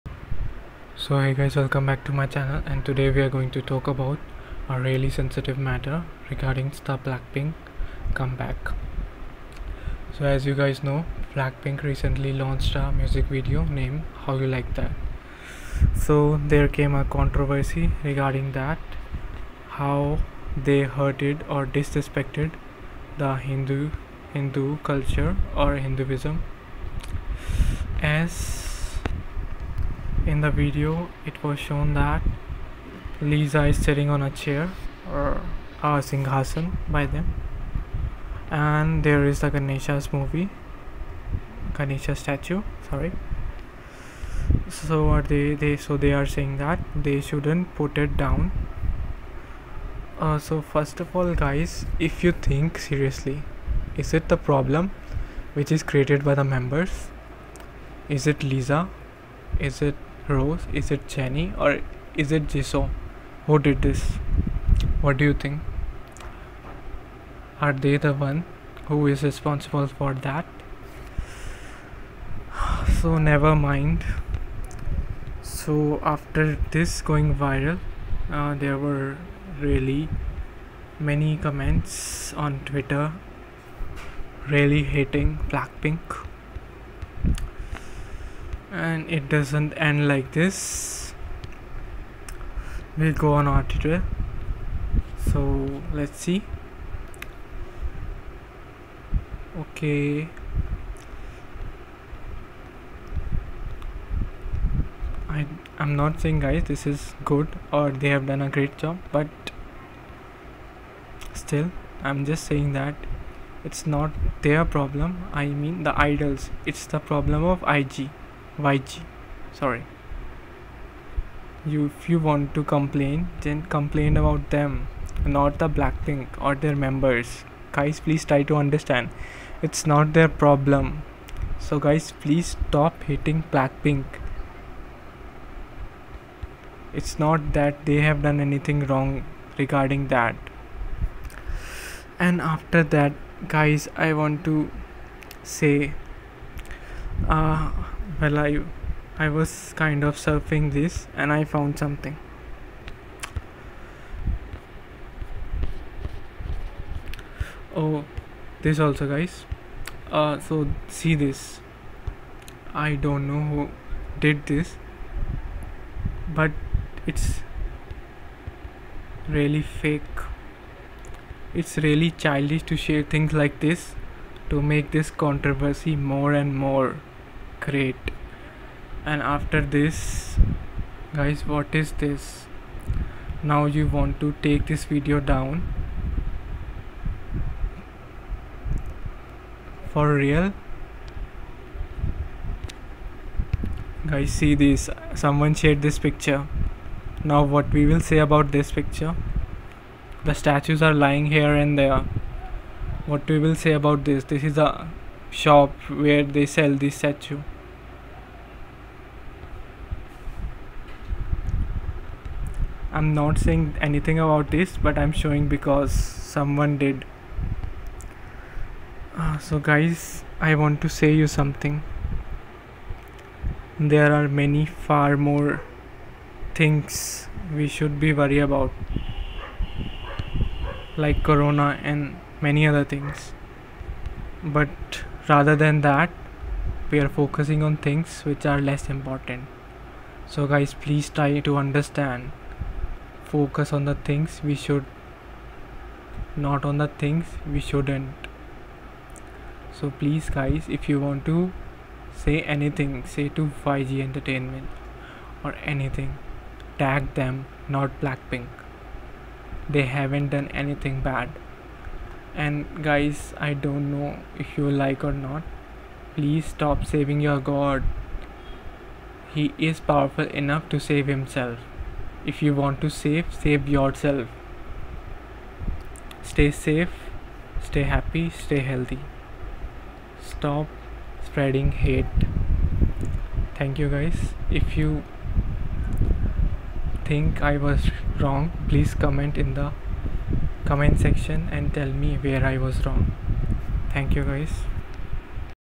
Yg should Apologize for hurting Hindu Culture not Blackpink ( so stop hating bp for no reason ) !!
Guys please understand what I want to say rather then getting hurt or hating me !

please understand and let's spread love rather then hate.

Proud Army 💜💜💜
Proud Blink ❤️❤️❤️
Proud Indian 💞💕💞

So don't think I'm being biased , if you still want to say something comment down